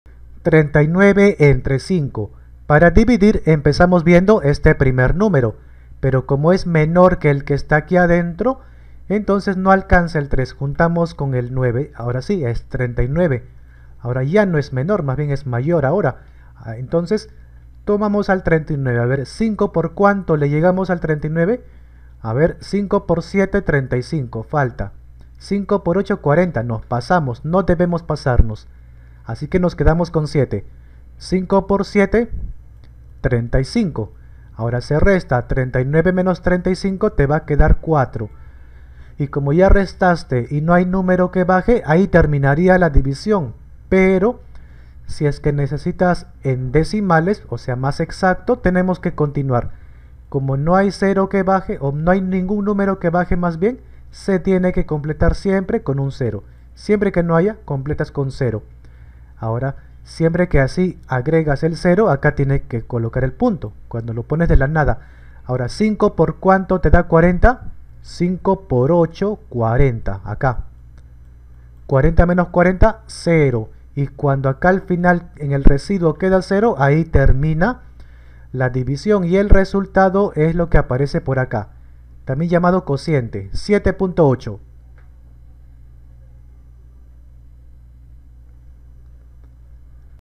Dividir 39 entre 5 , division inexacta con resultado decimal . Como se dividen 2 numeros
dividir hasta decimales
division con resultado decimal
como dividir
como se divide paso a paso